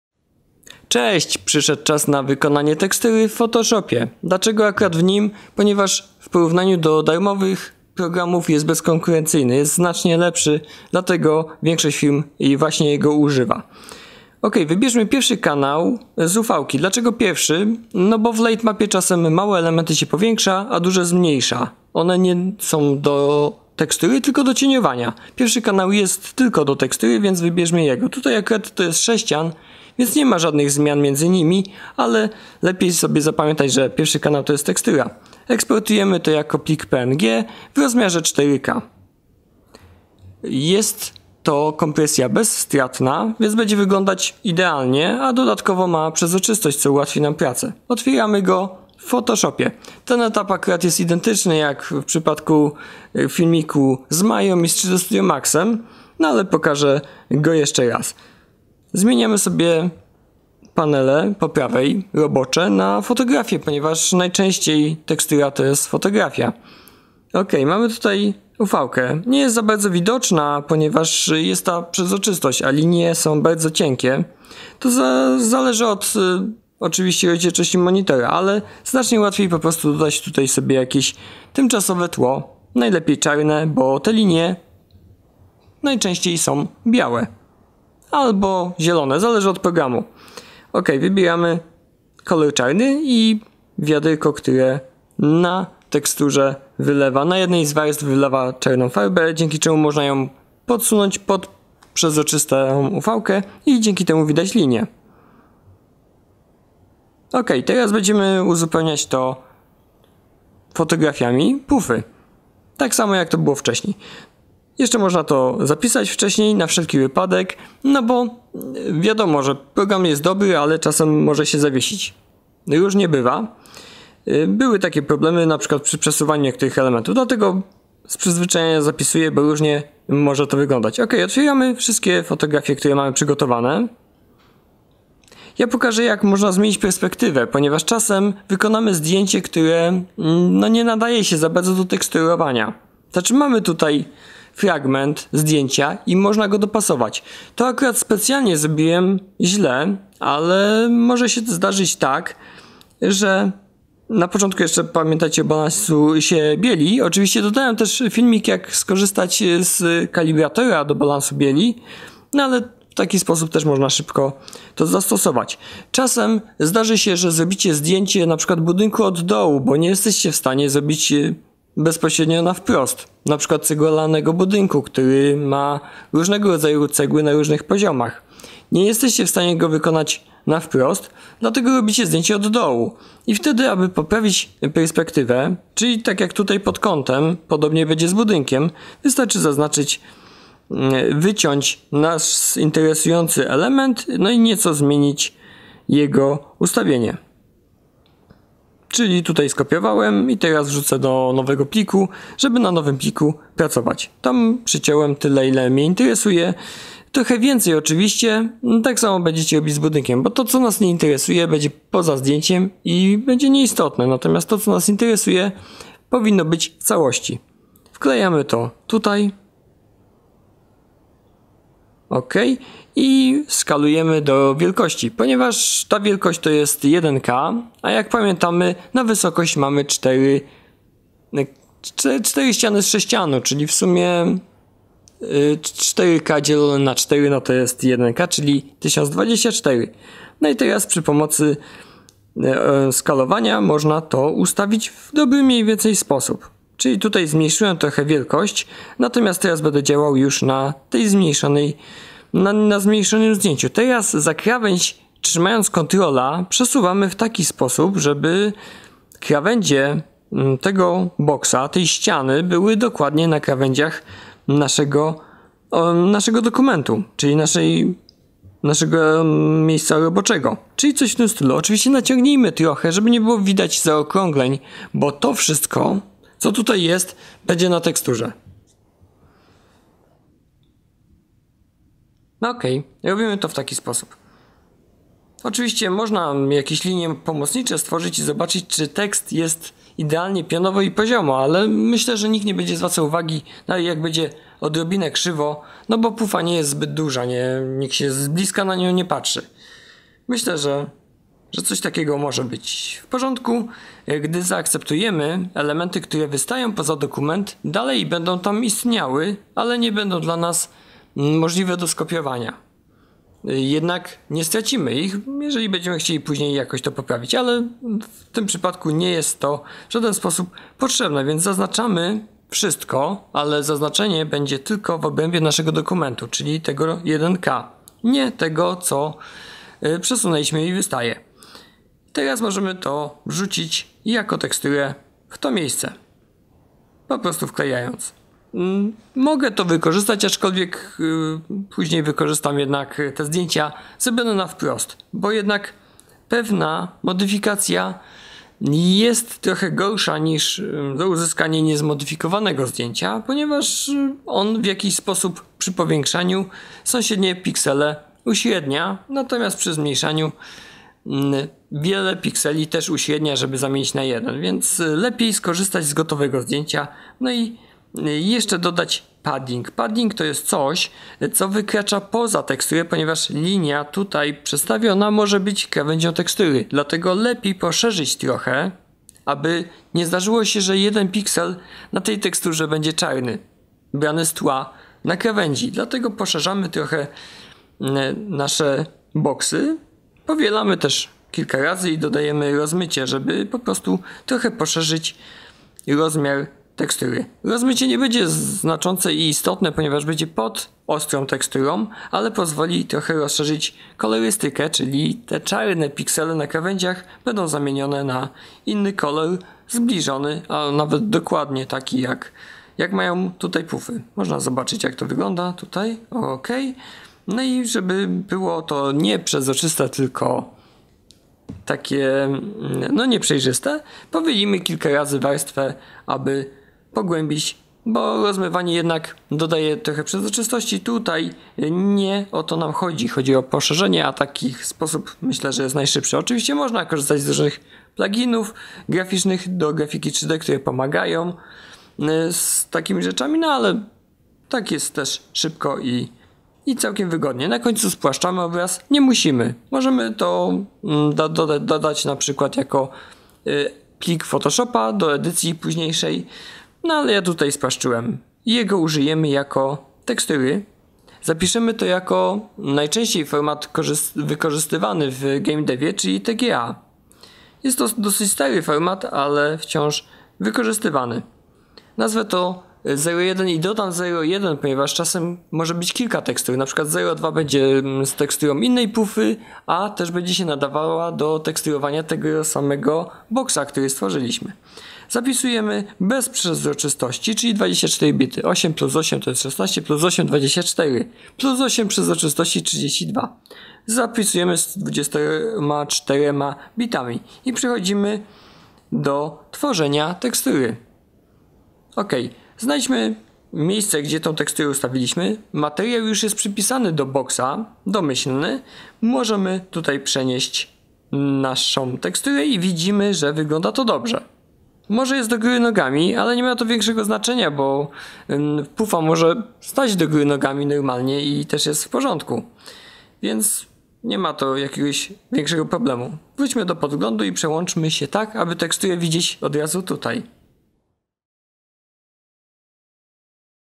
Uniwersytecki Puf - część 2 (Blender)
Druga część kursu pokazująca podstawy tworzenia tekstury obiektu do gry z wykorzystaniem Blendera i Adobe Photoshop.